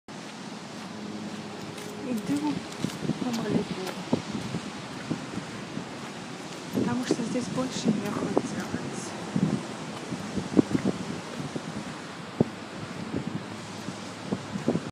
🚻 Иду по Малибу 🚽
Туалеты тут дерьмовые.